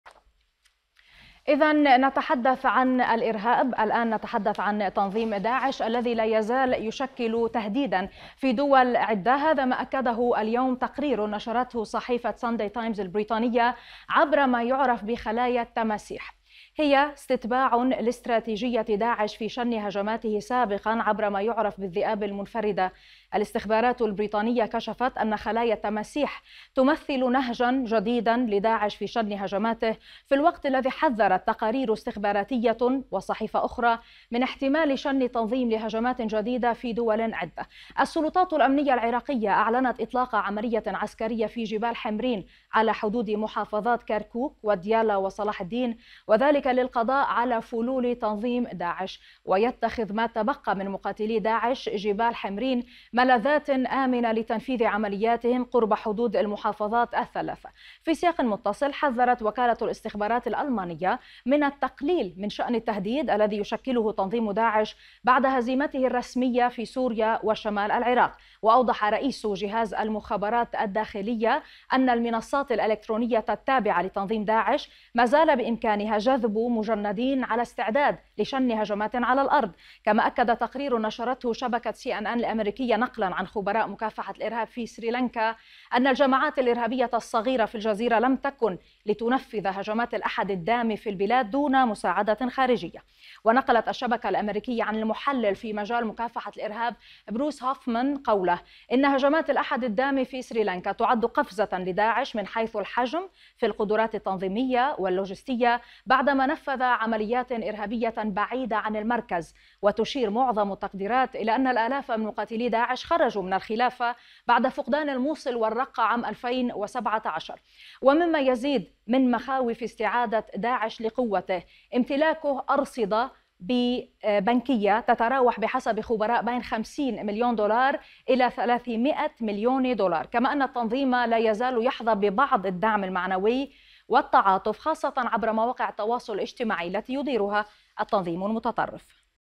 منها عبر خلايا التماسيح تحذيرات استخباراتية من هجمات جديدة لداعش
أخبار الآن | دبي - الإمارات العربية المتحدة (غرفة الأخبار)

لايزال تنظيم داعش يشكل تهديدا في دول عدة، وهو ما اكده تقرير نشرته صحيفة "صنداي تايمز" البريطانية، عبر ما يعرف بـ"خلايا التماسيح"، وهي استتباع لاستراتيجية داعش في شن هجماته سابقا عبر ما يعرف بـ"الذئاب المنفردة".

الاستخبارات البريطانية كشفت أن خلايا التماسيح تمثل نهجا جديدا لداعش في شن هجماته، في الوقت الذي حذرت تقارير استخباراتية وصحفية أخرى، من احتمال شن التنظيم لهجمات جديدة في دول عدة.

أعلنت السلطات الأمنية العراقية إطلاق عملية عسكرية في "جبال حمرين" على حدود محافظات كركوك وديالى وصلاح الدين، وذلك للقضاء على فلول تنظيم داعش.

ويتخذ ما تبقى من مقاتلي داعش، جبال حمرين، ملاذات آمنة لتنفيذ عملياتهم قرب حدود المحافظات الثلاثة.

في سياق متصل، حذرت وكالة الاستخبارات الألمانية من التقليل من شأن التهديد الذي يشكله تنظيم داعش بعد هزيمته الرسمية في سوريا وشمال العراق.
وأوضح رئيس جهاز المخابرات الداخلية، توماس هالدنفانغ، أن المنصات الإلكترونية التابعة لتنظيم داعش، مازال بإمكانها جذب مجندين على استعداد لشن هجمات على الأرض.

كما أكد تقرير نشرته شبكة السي ان ان الأمريكية، نقلاً عن خبراء مكافحة الإرهاب في سريلانكا، ان الجماعات الإرهابية الصغيرة في الجزيرة لم تكن لتنفذ هجمات الأحد الدامي في البلاد دون مساعدة خارجية.

ونقلت الشبكة الأمريكية عن المحلل في مجال مكافحة الإرهاب، بروس هوفمان، قوله، إن هجمات الأحد الدامي في سريلانكا تعد قفزة لداعش من حيث الحجم في القدرات التنظيمية واللوجستية بعدما نفذ عمليات إرهابية بعيدة عن المركز.

وتشير معظم التقديرات، إلى أن الآلاف من مقاتلي داعش خرجوا من الخلافة بعد فقدان الموصل والرقة في عام 2017.

ومما يزيد من مخاوف استعادة داعش لقوته، امتلاكه أرصدة بنكبة تتراوح بحسب خبراء بين 50 مليون دولار و كما 300 مليون دولار.

كما أن التنظيم لازال يحظى ببعض الدعم المعنوي والتعاطف وخاصة عبر مواقع التواصل الاجتماعي التي يديرها التنظيم المتطرف.